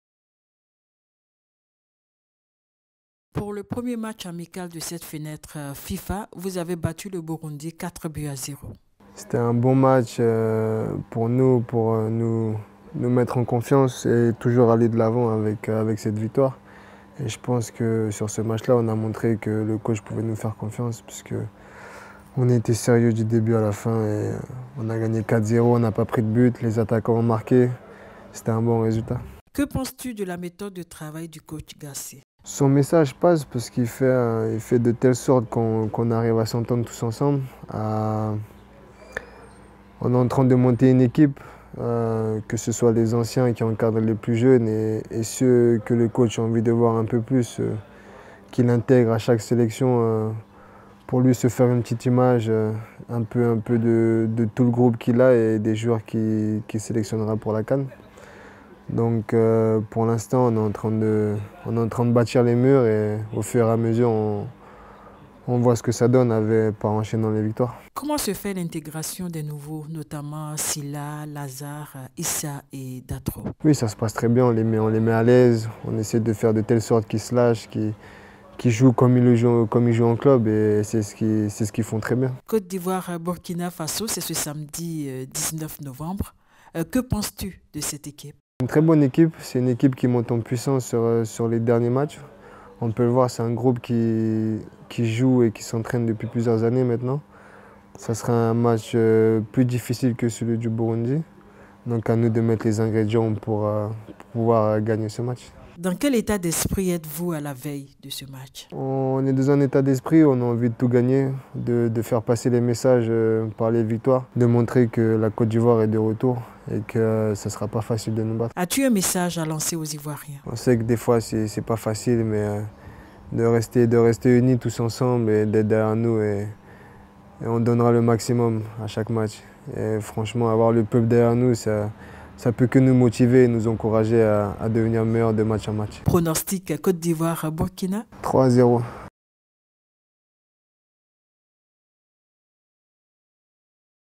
Matchs amicaux de novembre 2022 , méthode Gasset : Jean-Philippe Gbamin se prononce
Jean-Philippe Gbamin, né le 25 septembre 1995 à San-Pédro en Côte d'Ivoire, est un footballeur international ivoirien qui évolue au poste de défenseur ou de milieu défensif à Trabzonspor, en prêt d'Everton FC.